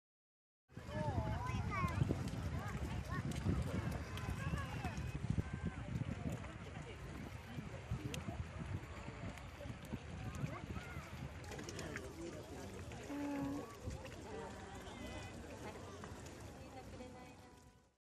【速報】東京湾で一足早い潮干狩り 千葉・富津、家族連れが熱中
春の訪れを告げる千葉県富津市の富津海岸潮干狩り場が19日、オープンした。富津漁業協同組合によると、東京湾沿いでは最も早く潮干狩りを楽しめるスポットの一つ。8月31日まで。